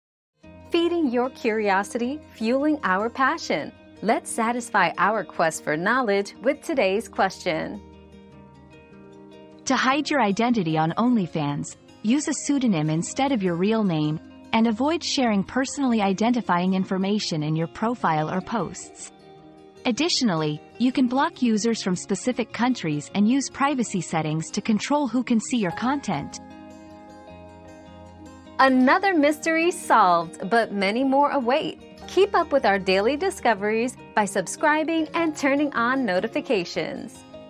How to hide your identity on OnlyFans?
Keep Your Identity Secret on OnlyFans! 👉 Secret Identity Tips 👉 Learn how to protect your privacy and keep your identity hidden on OnlyFans by using a pseudonym, avoiding personal information, blocking users, and adjusting privacy settings.

Laura S. Harris (2024, June 15.) How to hide your identity on OnlyFans?
    🌐 AskAbout.video